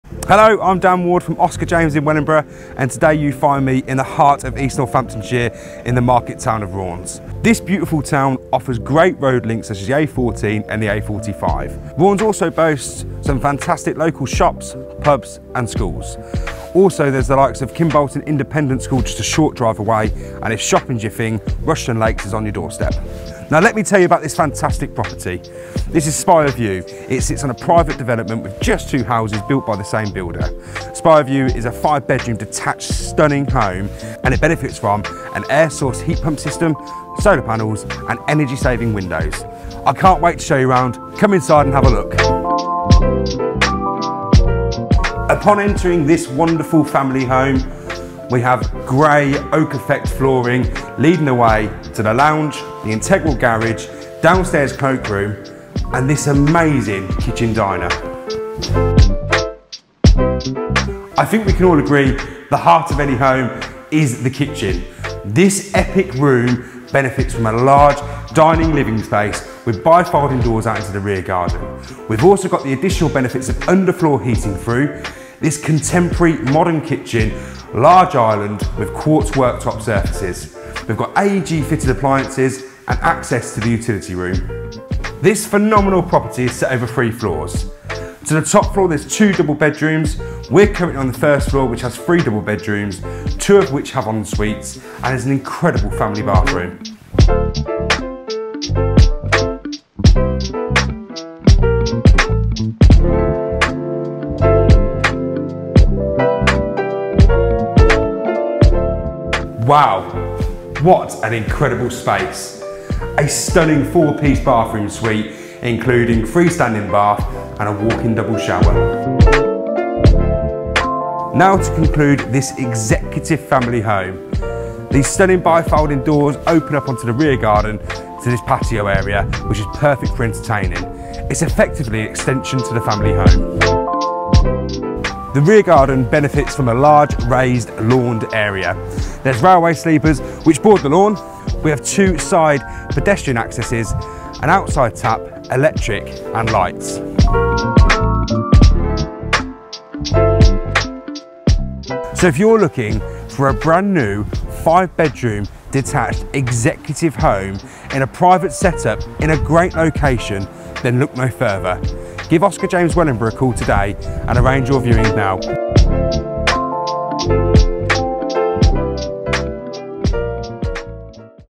Rotton Row, Raunds
Welcome to Spire View. A stunning executive family home in the Market town of Raunds. Standing proudly in an elevated position off the beaten track, this amazing five bedroom home offers over 2200 sqft of space across three floors.
This property has not only been well designed to suite modern family life but the quality of the craftsmanship throughout is for all to see, finished to the highest of standards, Spire View is the perfect family home.

Upon entering this impressive home, the spacious entrance hall is accessed via the composite door, the entrance hall is flooded with natural light from the full length obscure double glazed sidelight. The grey oak effect flooring runs through the entrance hall which gives access to, the lounge, integral garage, cloakroom & the stunning kitchen/diner. other benefits of the entrance hall include white painted staircase rising to the first floor. and the thermostat for the under floor heating.

The kitchen/diner is the real showstopper of this beautiful home, the grey oak effect flooring continues from the entrance hall to this unbelievable space. Finished to the highest standard with grey gloss wall and base units, with quartz work top surfaces over including the large island which takes centrepiece in the kitchen area. Fitted appliances include AEG double oven, AEG induction hob, NEFF dishwasher and integrated fridge/freezer, there is also a boiling hot tap. The kitchen/diner boast over 32ft in width also benefits from bi-folding doors into the rear garden.